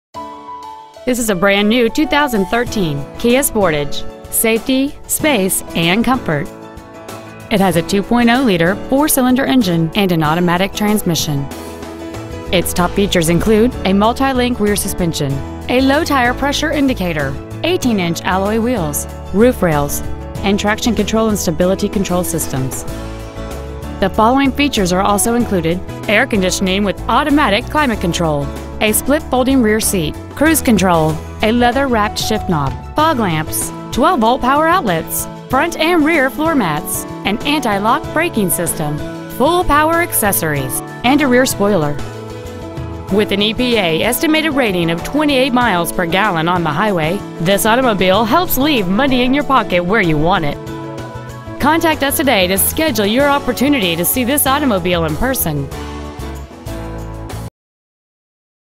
2013 Kia Sportage Austin TX
Year : 2013
 Make : Kia
 Model : Sportage
 Engine : 2.0L GDI turbo I4 engine
 Trans . : Automatic
 Exterior : Silver

 Interior : Black

 5306 South I H 35

 2013 Kia Sportage Austin TX